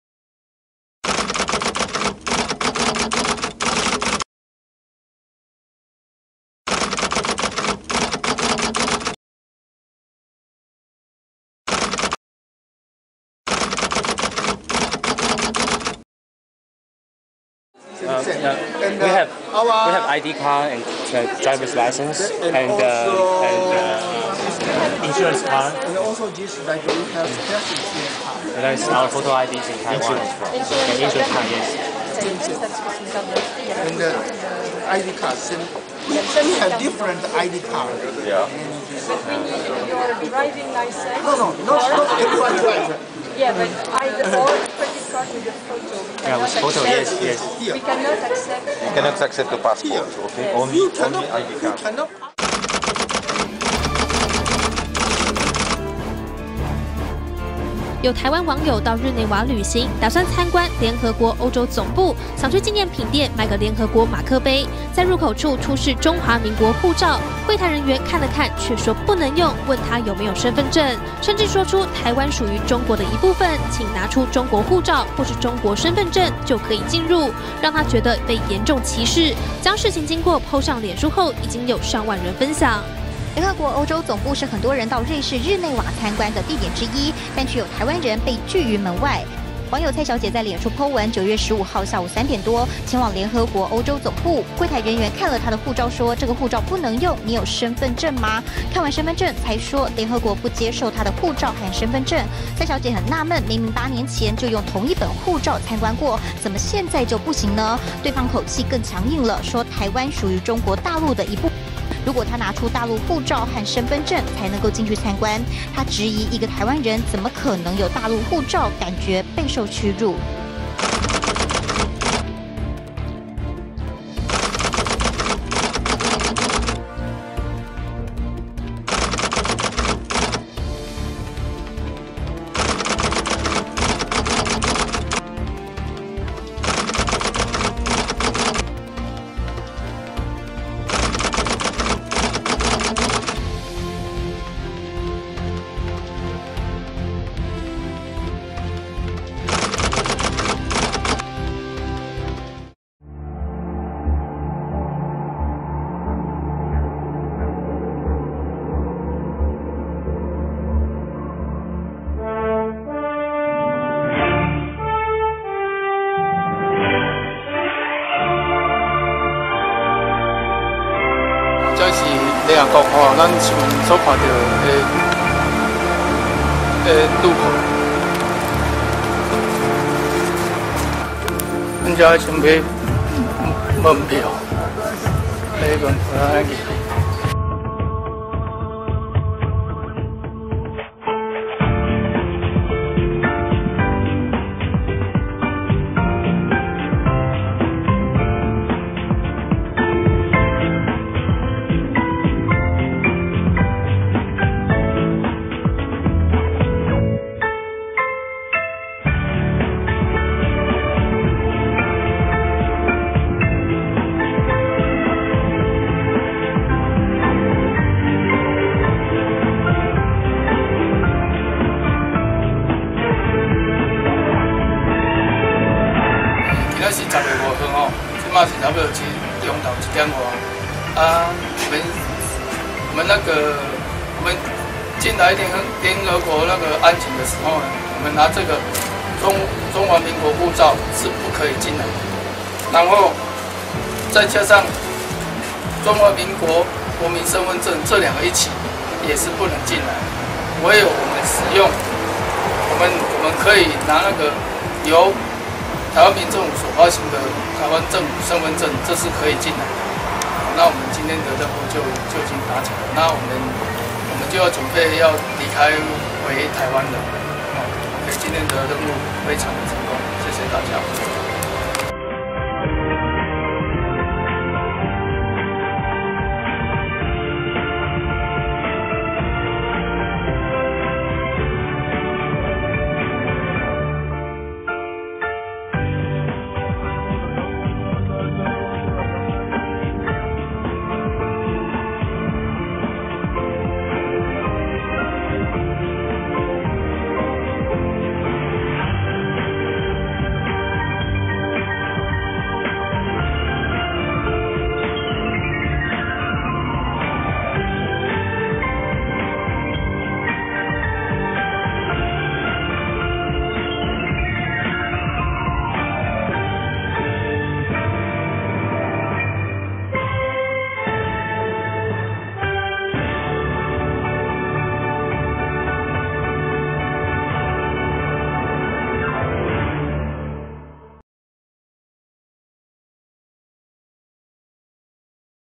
聯合國證實 台灣民政府 發行的身分證 是 台灣政府身分證 2015-10-5(12:07)
歡迎 中華民國流亡政府 駐外官員 集體申請 台灣民政府 台灣政府身分證